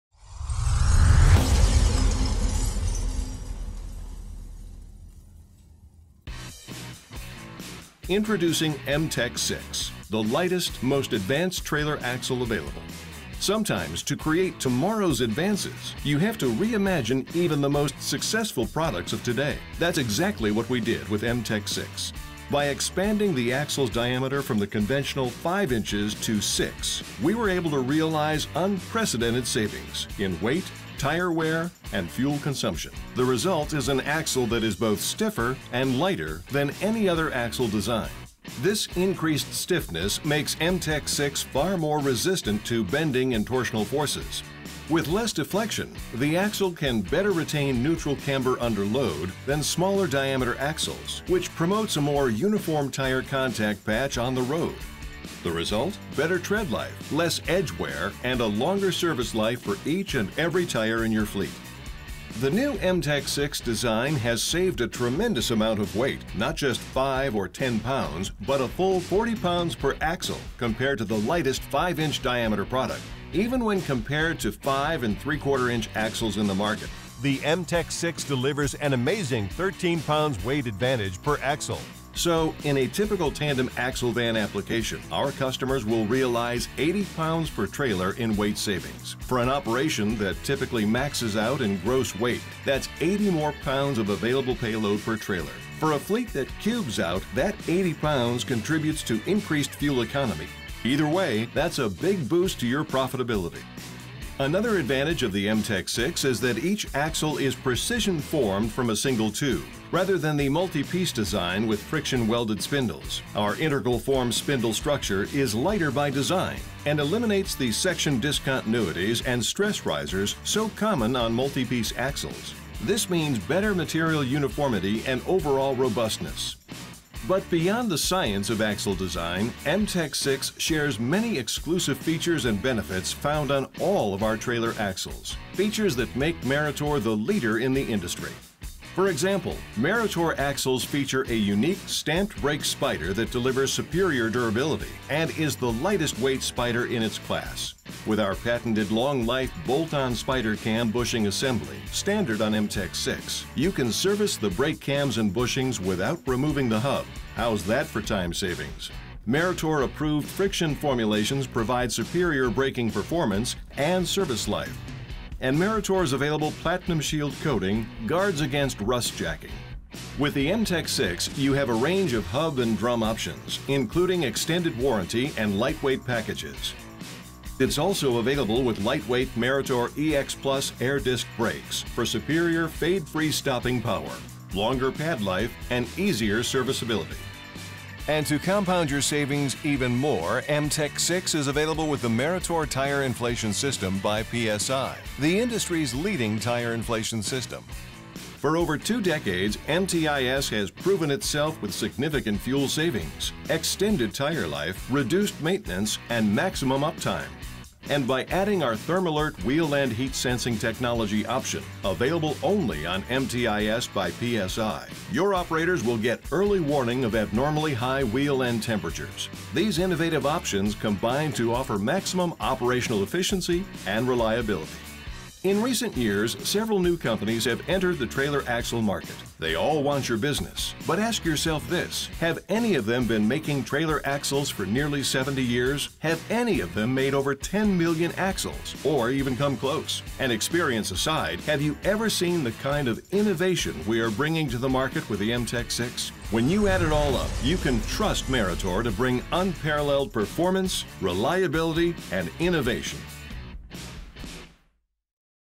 Meritor’s MTec6™ –The Industry’s Lightest Trailer Axle
Meritor's MTec6™, the industry's lightest trailer axle that offers greater weight savings and lower total cost of ownership.